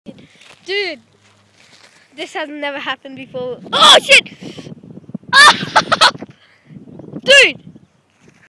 Kid falls of a water tank and gets knocked out!!!
THIS IS TOTALLY LEGIT.. HE FELL OFF AND GETS KNOCKED OUT!!! LOL